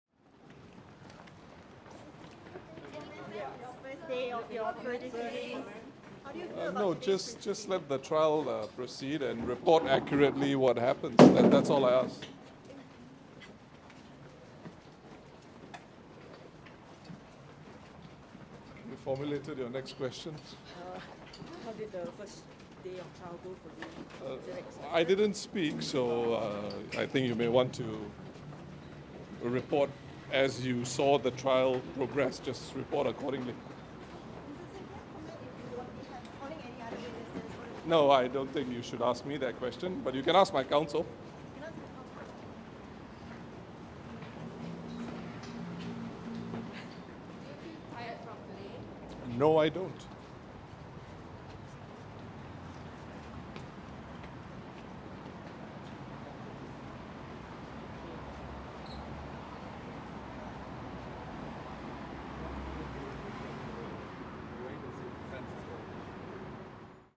Pritam leaves court after first day of trial
Workers’ Party chief and Leader of the Opposition Pritam Singh left the State Courts, ending his first day of trial with testimony from former WP MP Raeesah Khan.

Singh, the secretary-general of the Workers’ Party, is expected to contest two charges over alleged lies to a parliamentary committee convened in November 2021 to look into the lying controversy involving Ms Khan. His trial is set to last for 16 days, concluding on Nov 13.